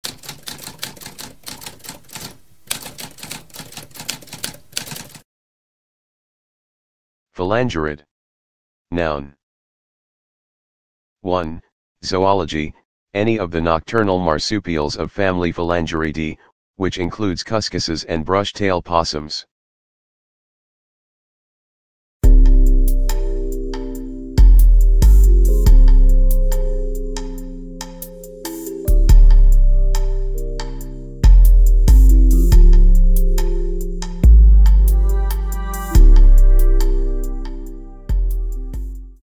What does phalangerid mean?
A spoken definition of phalangerid.

Intro Sound:
Typewriter - Tamskp